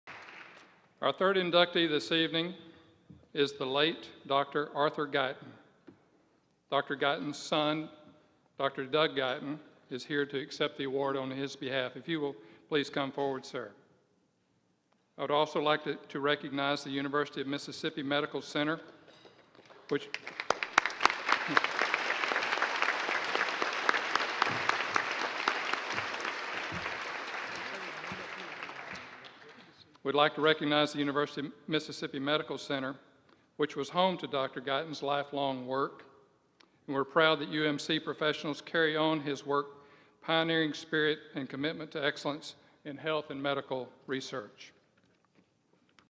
Dr. Arthur Guyton - 2008 Innovators Hall of Fame Excellence Award Recipient
Dr. Arthur Guyton, posthumously, author of the textbook of Medical Physiology, received the Excellence Award at the Mississippi Technology Alliance's Innovators Hall of Fame Awards Gala in 2008. Accepting the award, Dr. Doug Guyton, his son.